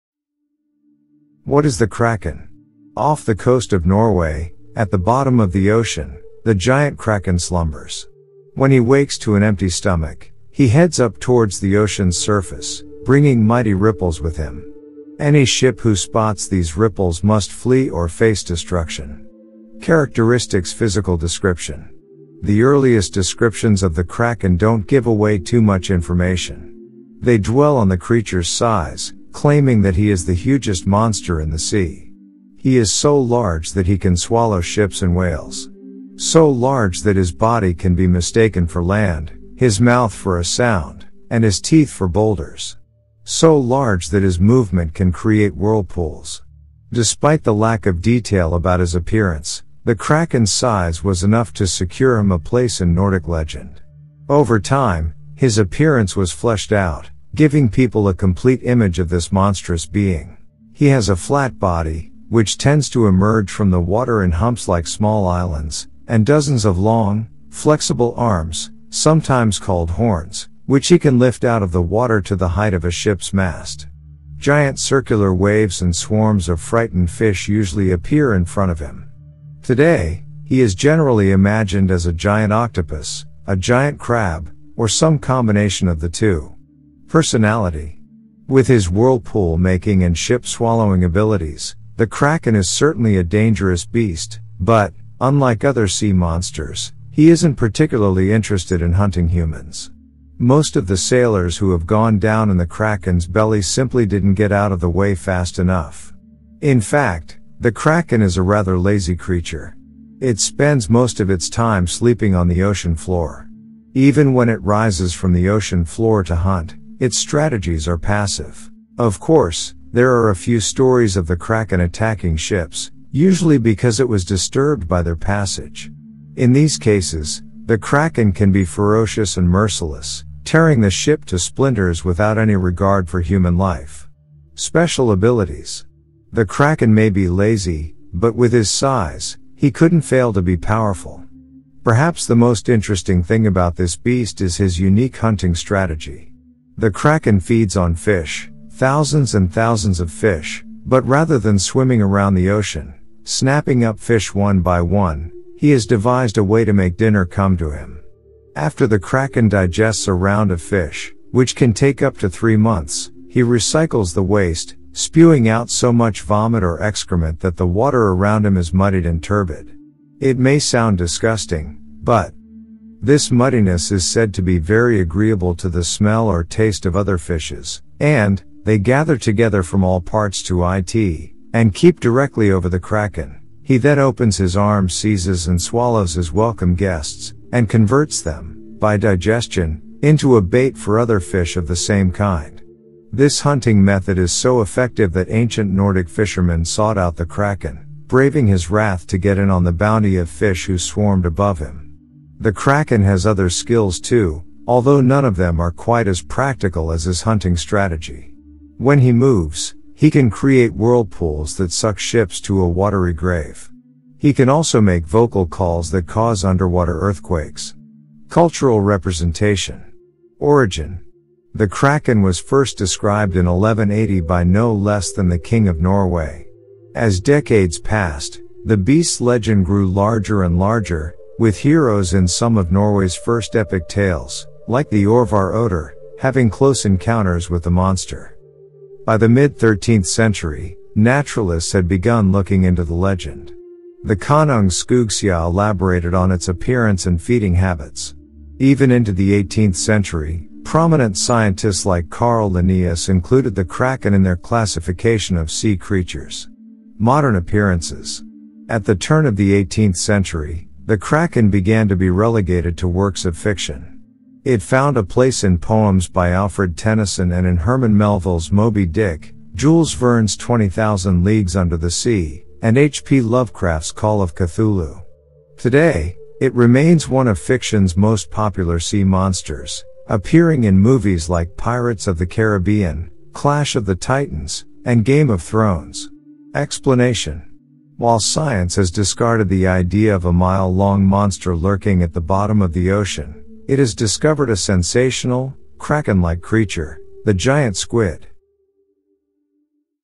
Kraken: The Legendary Sea Monster - Mythology and Folklore
The Legendary Kraken: The Real Animal Behind the Monster. Stories generally depict him as a very large octopus or squid-like creature that attacks ships. According to some stories, the Kraken is so large that its body can be mistaken for an island.